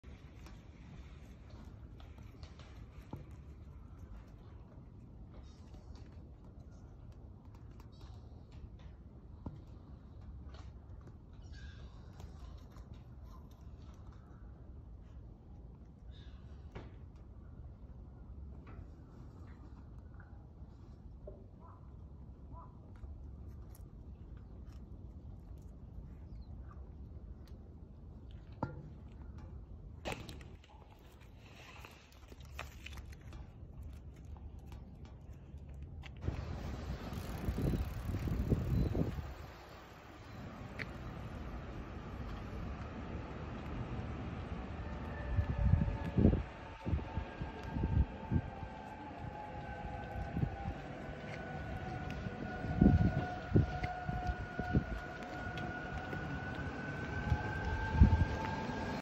This Nara Deer has lost her leg due to a car accident. Please drive slowly near Nara Park like this.
Lots of Nara Deer get hit by cars in and around Nara Deer Park and they either die or lose their leg.

Look at a female deer that lost her back leg.
She lost her mobility and she’d have to spend the rest of her life in this deer enclosure depriving her of freedom she was supposed to enjoy.

Dear drivers, please please drive slowly and carefully.

You would freak out when your family or friends get hit by cars.